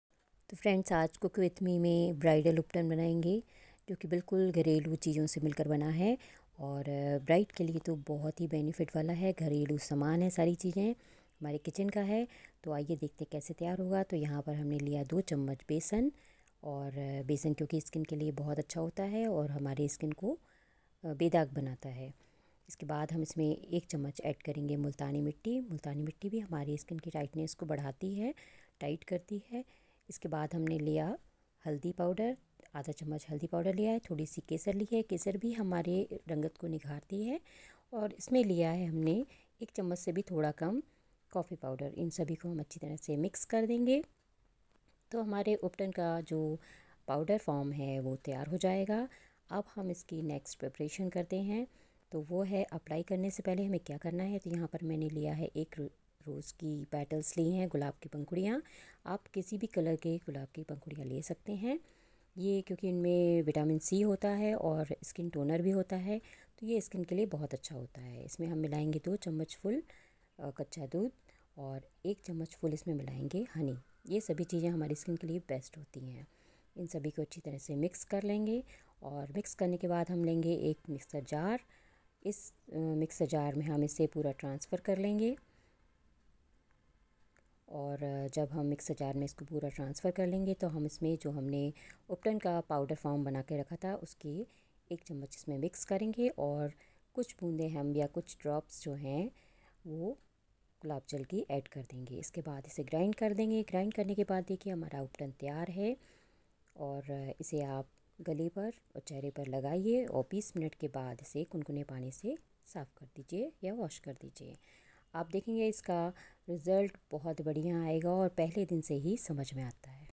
दुल्हन का उबटन बनाने की विधि / केवल रसोई सामग्री से महेंगे facial sa निखार@cookwithme1919
Ingredients

gram flour
Fuller's earth ( multani mitti )
turmeric powder
saffron
coffee powder
rose petals
rose water
honey

this remedy is very good for face